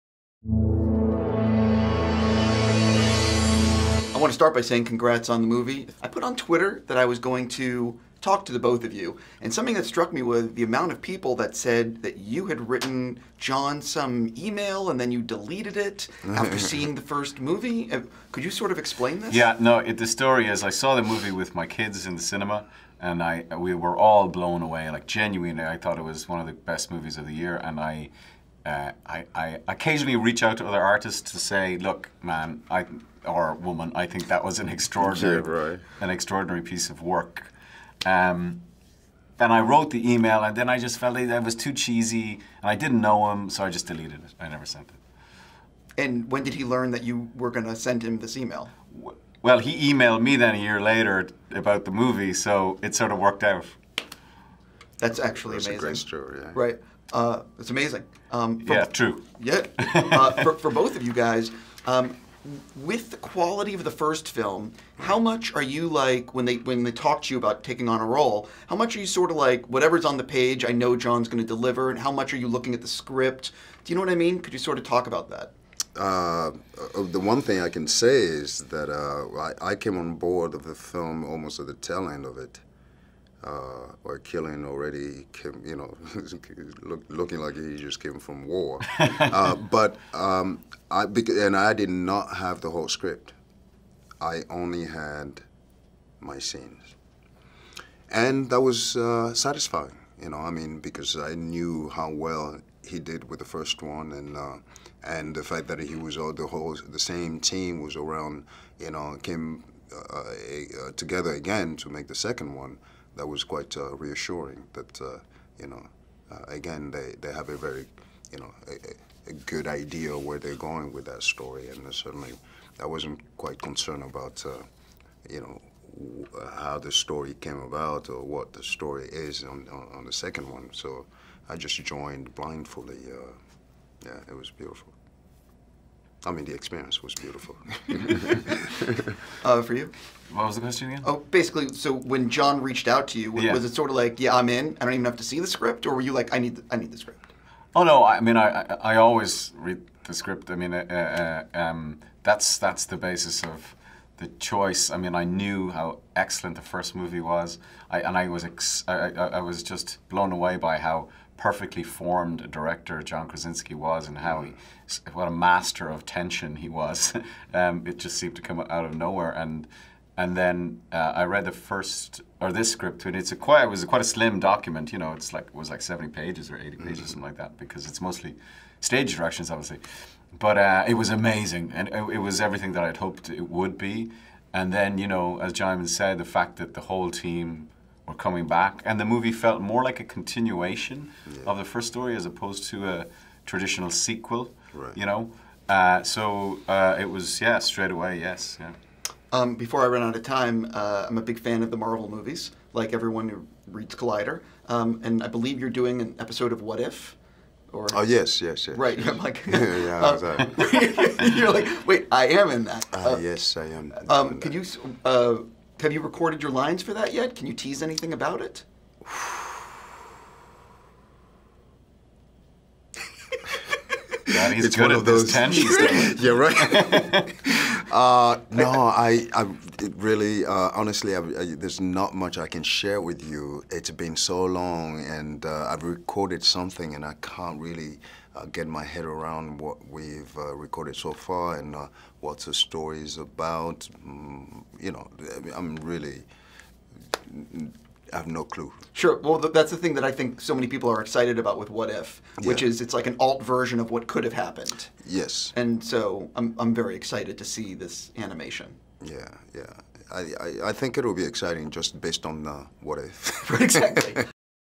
Cillian Murphy and Djimon Hounsou on ‘A Quiet Place Part II’ and Marvel’s ‘What if…?’
In March of 2020, right before COVID shut down the world, I traveled to New York City to see a screening of A Quiet Place Part II and then interviewed the cast and writer-director John Krasinski. At the time, people weren’t sure how this new mysterious virus was spreading, which meant when we sat down to do interviews, we were asked not to shake hands with anyone. We had no idea just being in the same room with someone infected would spread the disease.

Anyway, with A Quiet Place Part II finally set to be released in theaters this weekend, it’s finally time to share the interviews I conducted over a year ago.

During my conversation with Cillian Murphy and Djimon Hounsou, they talked about making the film, how Hounsou never read the whole script, why Murphy wrote Krasinski an email saying how much he loved the first movie after he saw it and why he never hit send, and more. In addition, with Hounsou voicing his MCU character Korath in an episode of Marvel's upcoming animated series, What If...?, I tried to get him to share some information about the myserious series.

If you haven’t seen the trailers, A Quiet Place Part II picks up almost immediately after the end of the first film and we find Emily Blunt's Evelyn Abbott and her kids (Millicent Simmonds and Noah Jupe) on the road searching for a way to survive. The sequel adds Murphy as a family friend and Hounsou as another survivor.

While we have all seen countless sequels that should have never been made, A Quiet Place Part II is not one of them. Krasinski has crafted a fantastic, nail-biting, edge of your seat thriller that absolutely should be seen in a theater (assuming you’ve been vaccinated). Trust me, this is a great movie.

Check out what Cillian Murphy and Djimon Hounsou had to say in the player above and below is exactly what we talked about.

Cillian Murphy and Djimon Hounsou:
    Murphy talks about how he was going to write John Krasinski an email saying how much he loved the first movie after he saw it and why he never hit send.
    Hounsou talks about how he never had the whole script.
    Murphy talks about his reaction to reading the script for the first time.
    I try and get Hounsou to talk about Marvel’s What If…?